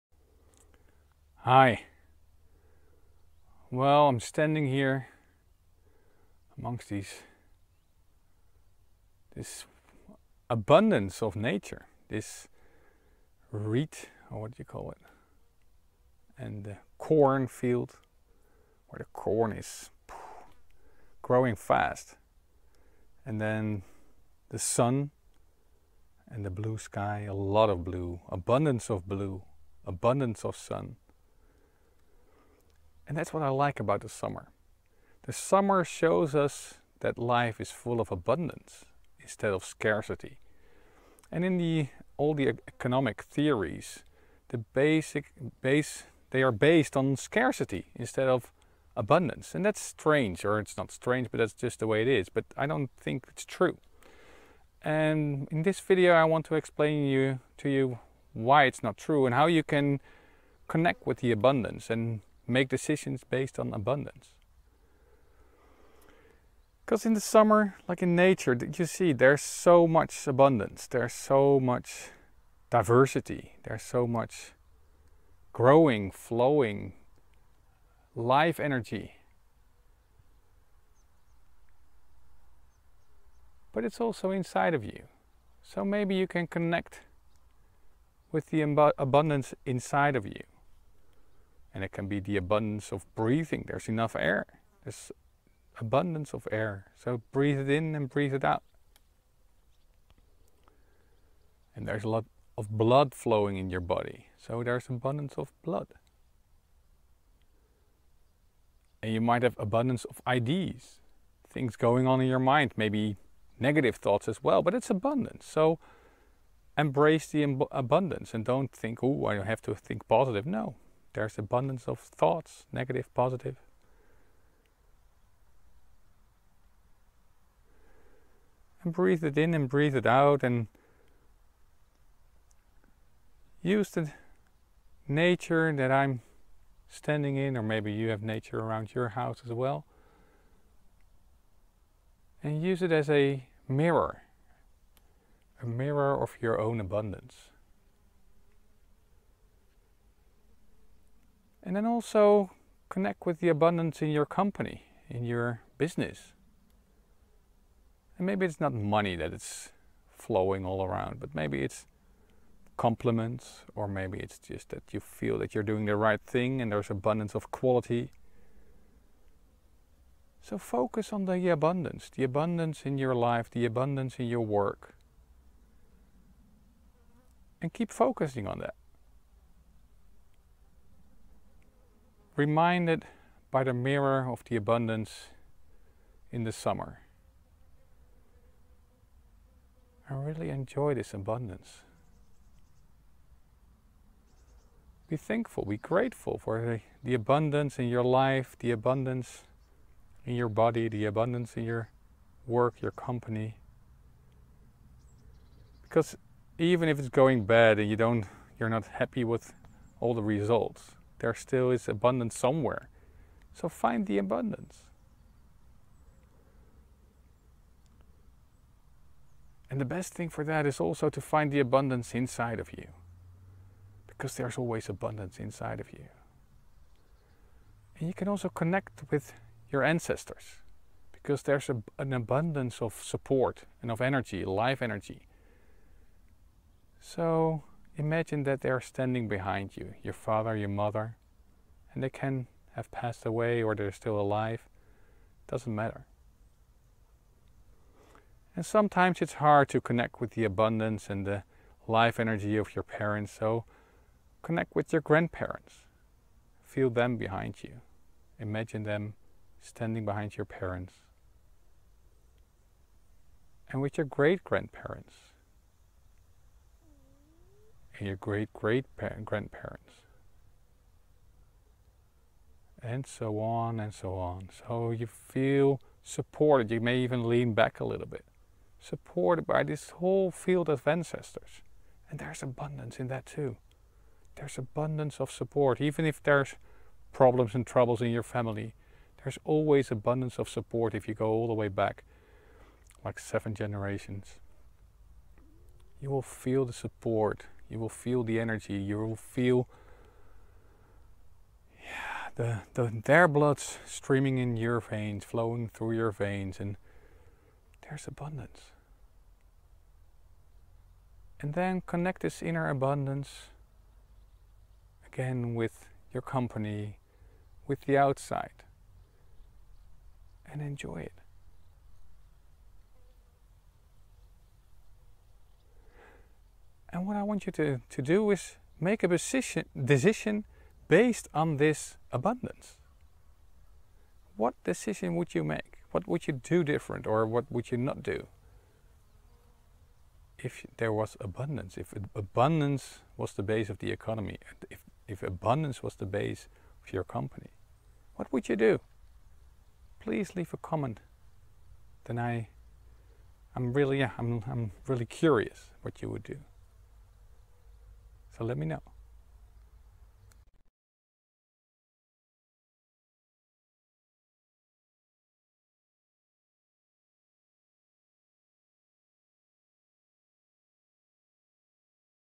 Are your actions based on abundance?
Martijn Meima, business coach and business intuition expert, tells more about how to act based on abundance in your company.